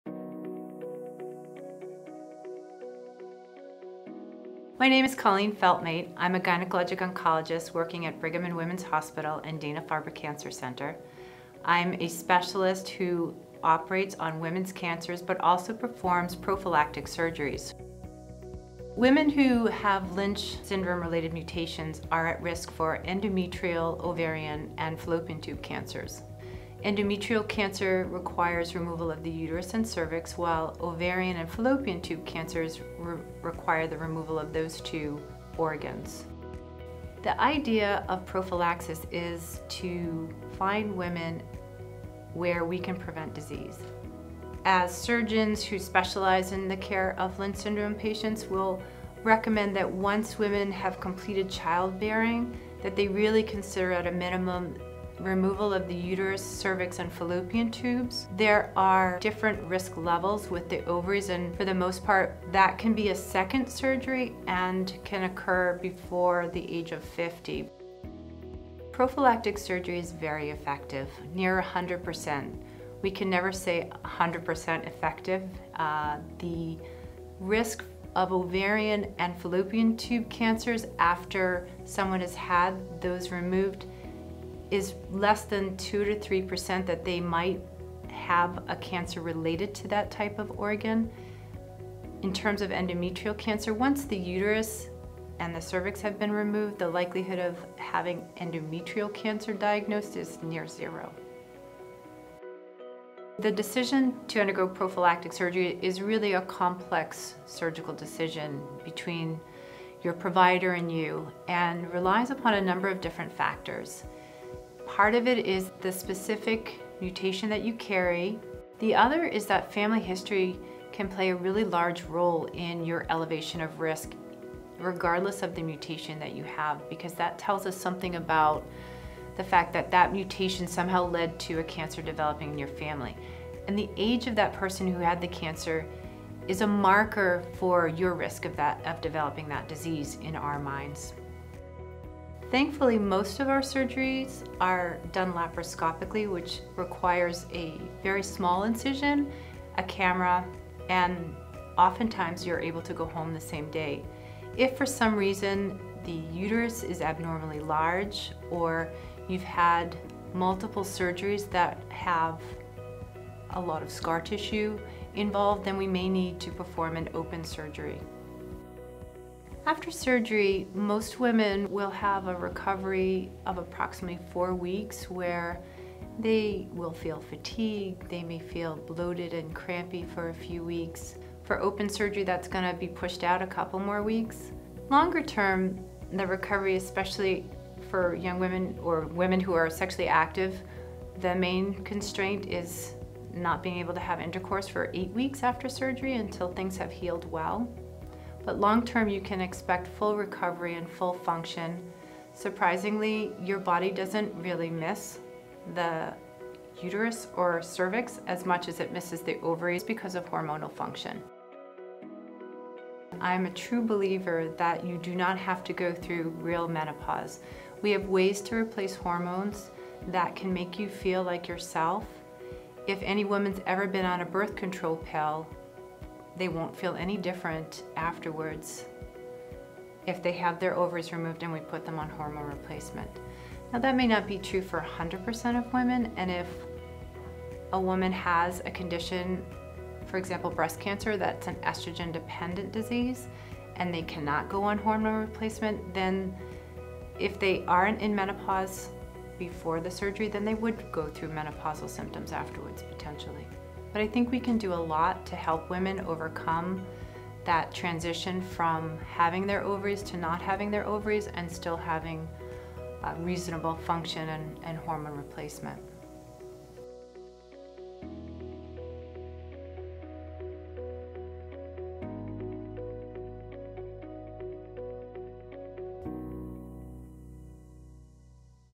Risk-Reducing (Prophylactic) Gynecological Surgery in Lynch Syndrome | Dana-Farber Cancer Institute
Colleen Feltmate, MD, a gynecologic oncologist at Dana-Farber Brigham Cancer Center, operates on women with gynecologic cancers and also performs prophylactic surgeries. She describes the types of gynecologic cancers that women with Lynch syndrome are at risk for, and the decision-making process for undergoing risk-reducing (prophylactic) surgery to prevent cancer.